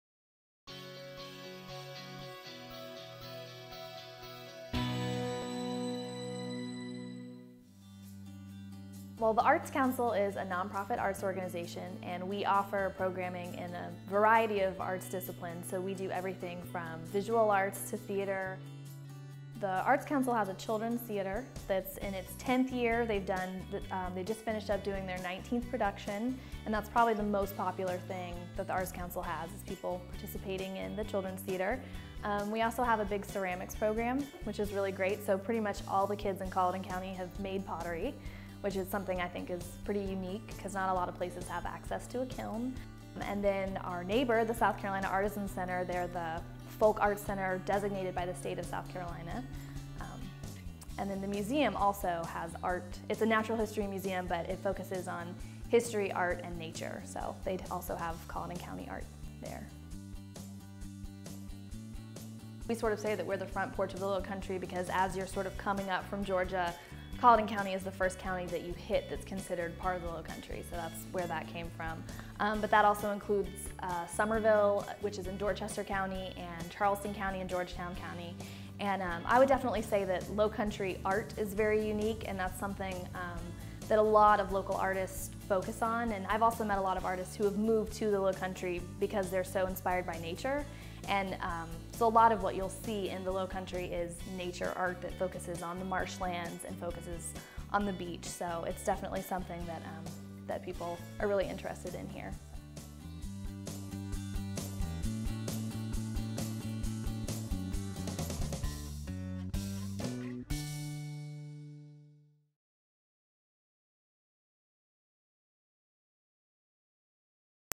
Colleton County Arts
Lowcountry art is just part of what makes Colleton County such a unique place to visit. The county is home to the South Carolina Artisan Center, and the Arts Council provides a myriad of opportunities for the community to explore the arts.